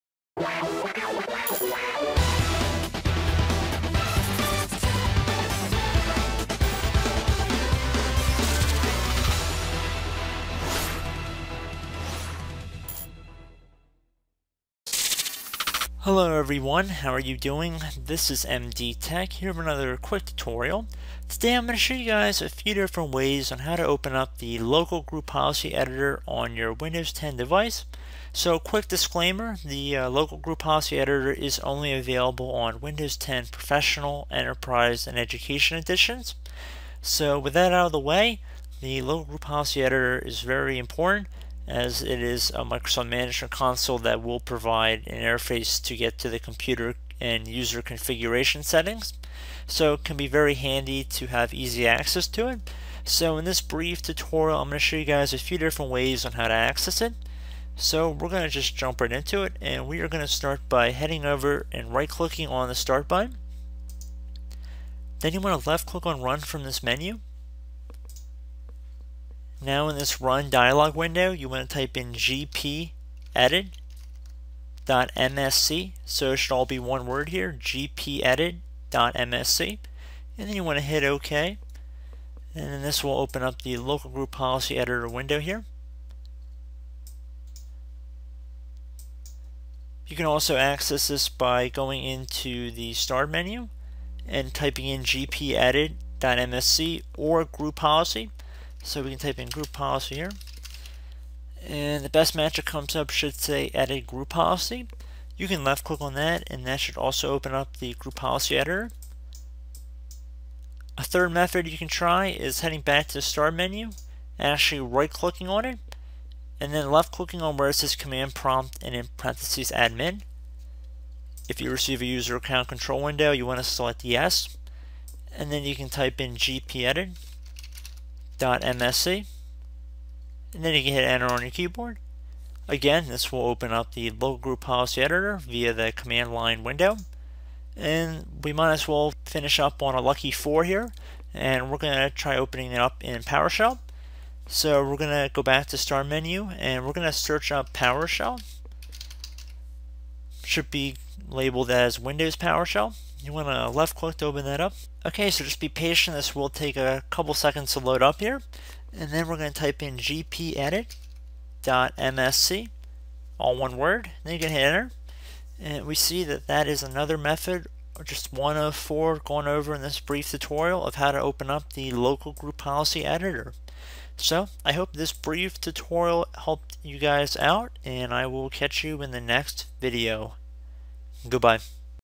How To Open Local Group Policy Editor (gpedit msc) On Windows 10/8/7
This tutorial will show you different ways on how to open the Local Group Policy Editor in Windows 10.

Please note the Local Group Policy Editor is not available in Windows 10 Home edition.